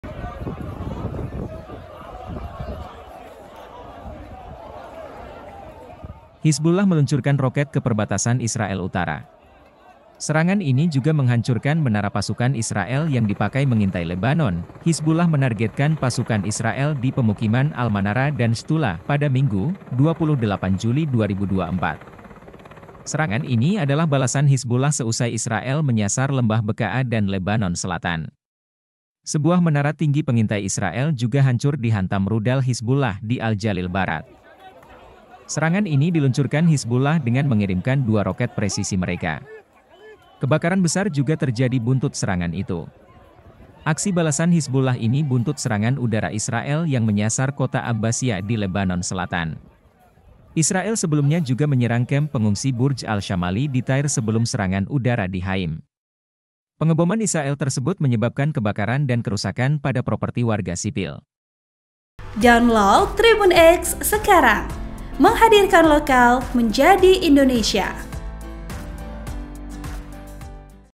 BUTAKAN ISRAEL! Roket Ganas Hizbullah Hancurkan Menara Intai Tentara Israel
Editor Video: Rizky Puji

TRIBUNJATIM.COM - Hizbullah meluncurkan roket ke perbatasan Israel Utara.

Serangan ini juga menghancurkan menara pasukan Israel yang dipakai mengintai Lebanon.
Hizbullah menargetkan pasukan Israel di pemukiman Al-Manara dan Shtula pada Minggu (28/7/2024).

Serangan ini adalah balasan Hizbullah seusai Israel menyasar Lembah Bekaa dan Lebanon Selatan.

Sebuah menara tinggi pengintai Israel juga hancur dihantam rudal Hizbullah di Al-Jalil Barat.

Serangan ini diluncurkan Hizbullah dengan mengirimkan dua roket presisi mereka.

Kebakaran besar juga terjadi buntut serangan itu.

Aksi balasan Hizbullah ini buntut serangan udara Israel yang menyasar Kota Abbasiya di Lebanon Selatan.

Israel sebelumnya juga menyerang kamp pengungsi Burj Al-Shamali di Tyre sebelum serangan udara di Khaim.

Pengeboman Israel tersebut menyebabkan kebakaran dan kerusakan pada properti warga sipil.